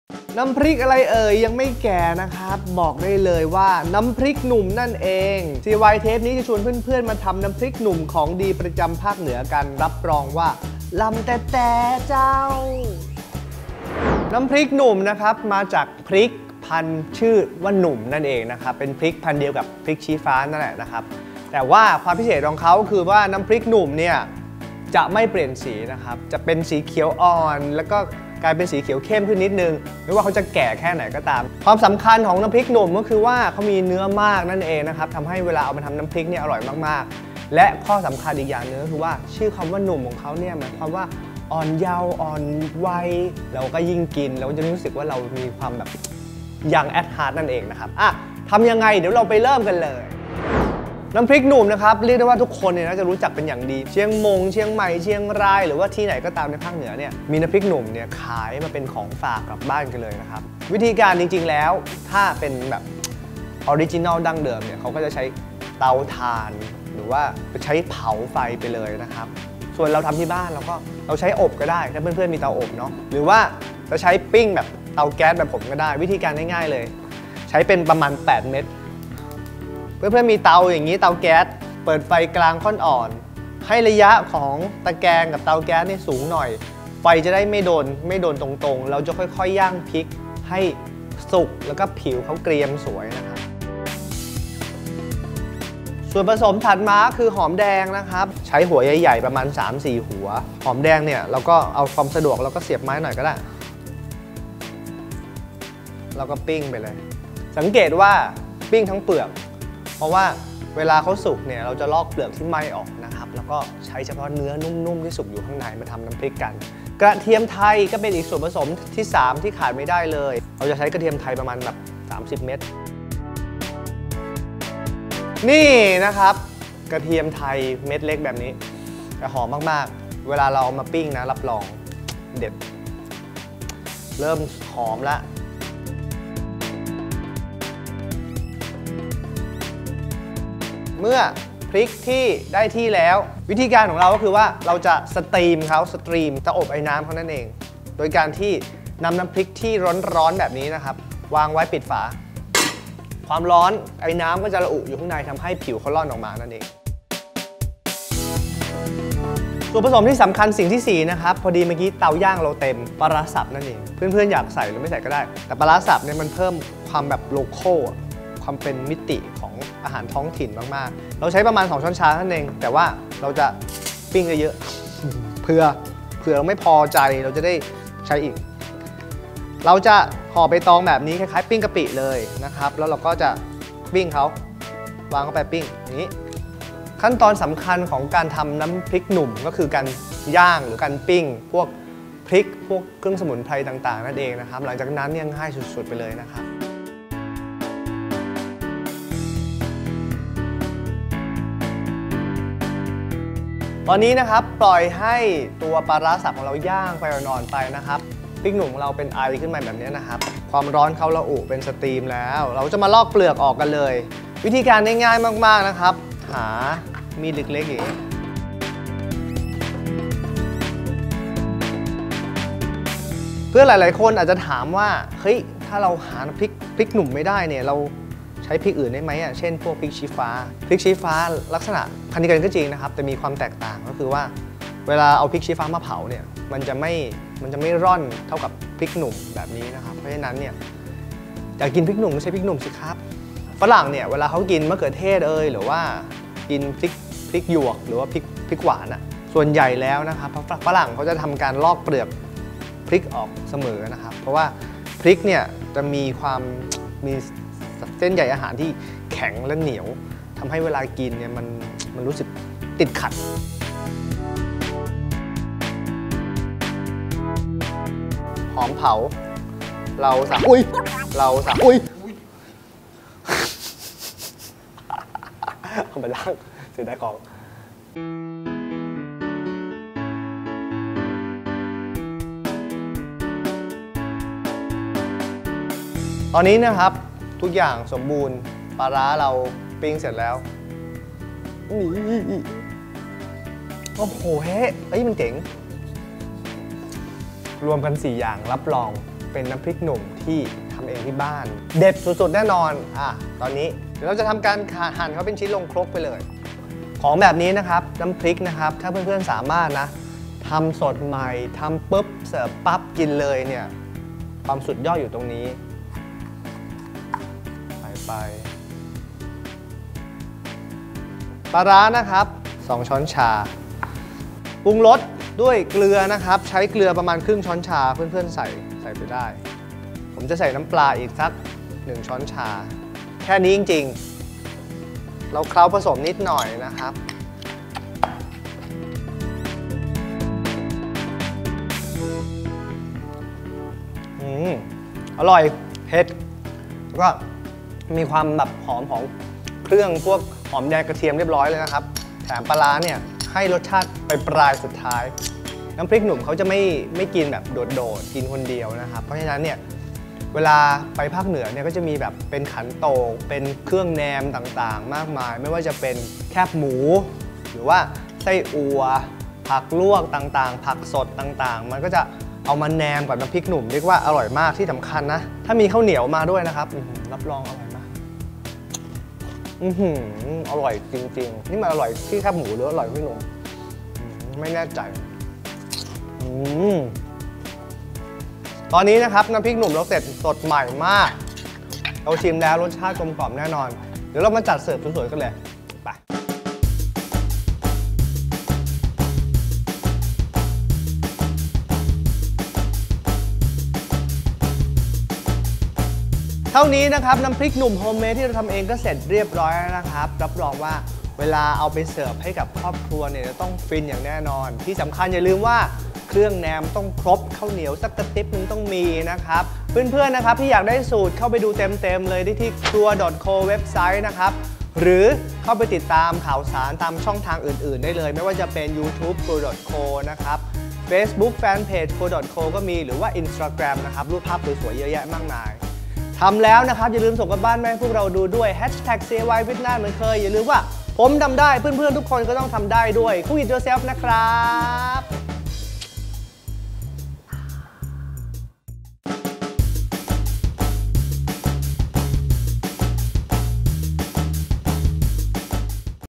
น้ำพริกหนุ่ม อาหารเหนือ ทำง่ายๆ ได้ที่บ้าน - by เชฟน่าน | CIY - Cook it your self
สัปดาห์นี้ CIY ขอเสนอ น้ำพริกหนุ่ม น้ำพริกสูตรเด็ดของเมืองเหนือที่คนทุกภาคชื่นชอบ วิธีทำก็ไม่ยากวัตถุดิบก็หาง๊ายง่าย  จัดเสิร์ฟพร้อมแคปหมูไส้อั่ว เครื่องแนมอื่นๆตามใจชอบ รับรองว่าลำแต้แต้เจ้า